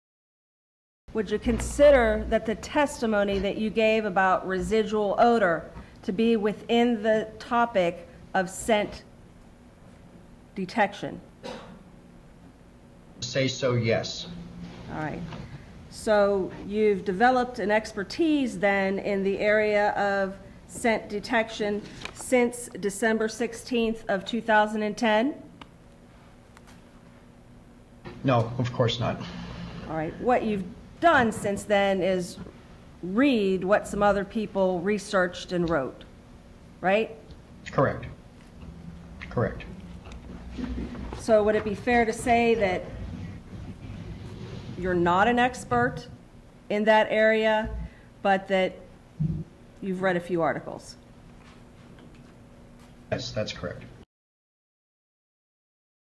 FAIRGRIEVE//NOT EXPERT
Shot 03/23/2011. day 1 of a 3 day Motions hearing in the Casey Anthony murder trial--Casey Anthony is charged with first-degree murder in the 2008 death of her 2-year-old daughter, Caylee Marie. She has pleaded not guilty, claiming a babysitter kidnapped her toddler. Caylee was last seen in June of 2008, but she was not reported missing until a month later. Her remains were found in woods near the family's home in December 2008. PROFESSOR SCOTT FAIRGRIEVE AND PROSECUTOR LINDA DRANE BURDICK day 1 of a 3 day Motions hearing in the Casey Anthony murder trial FOR IN SESSION 15:37:29 WOuld you consider 15:38:22 yes, that's correct.